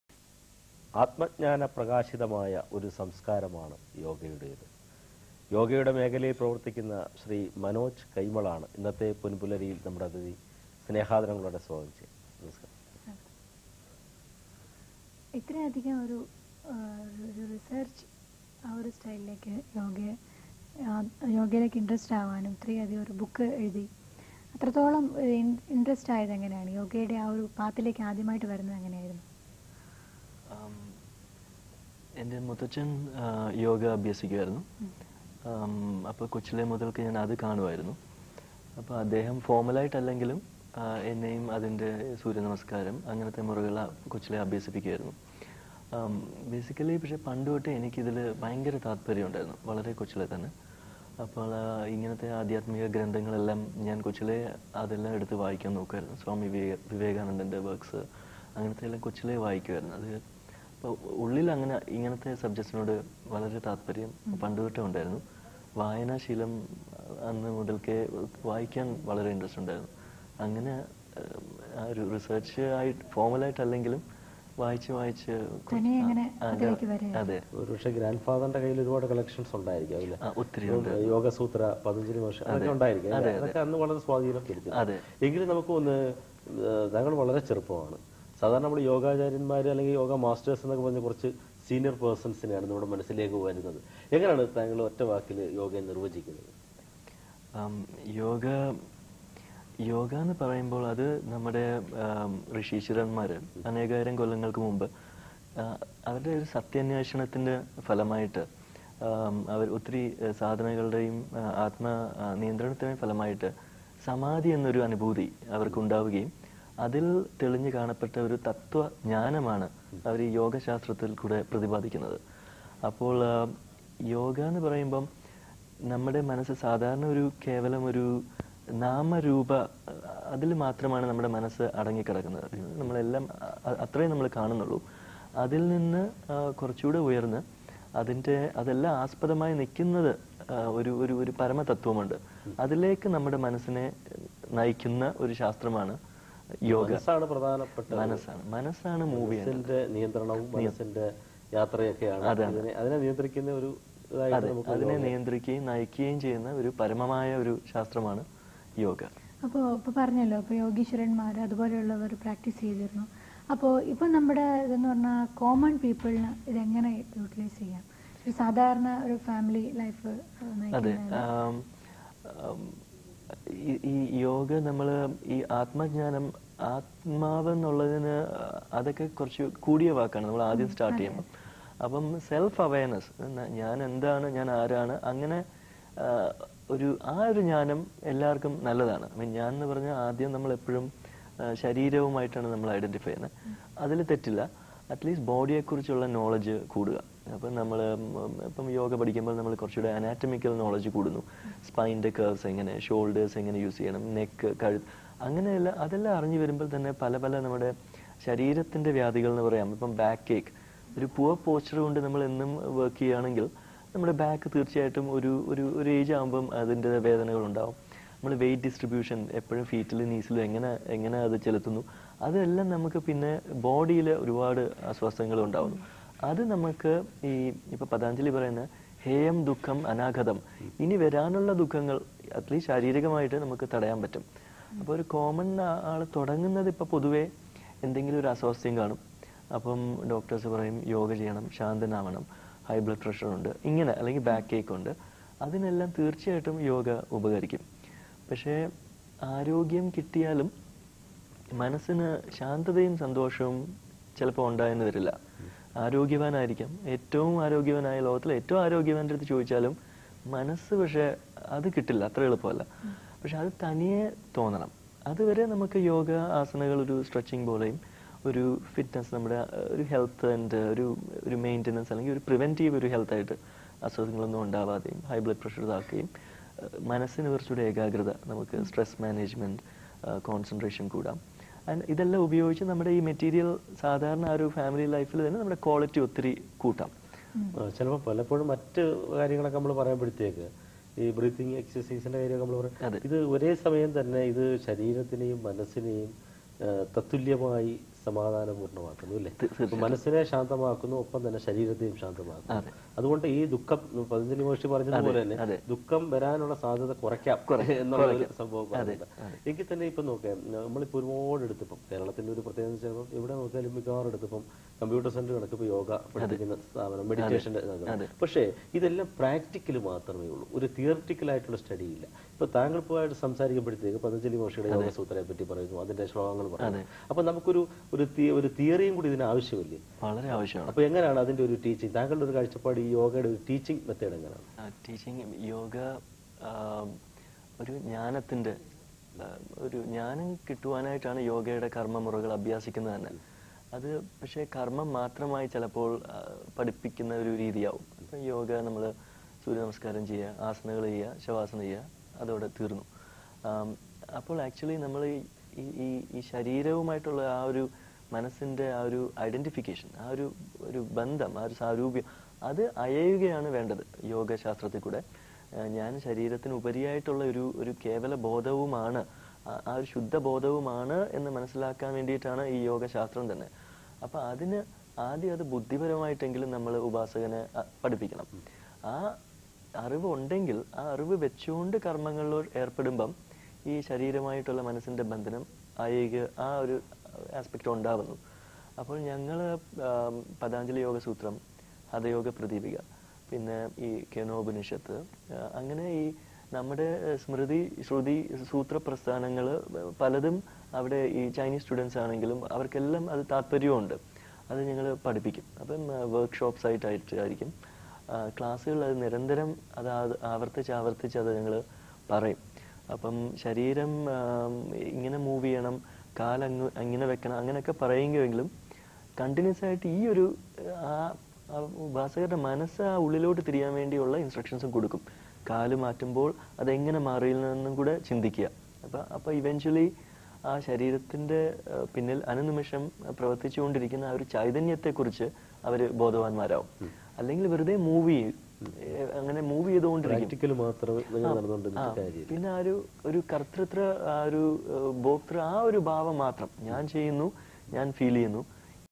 Manoj Kaimal's Talk on Surya TV, Kerala in Malayalam Part 1/3
Manoj Kaimal Talks about Yoga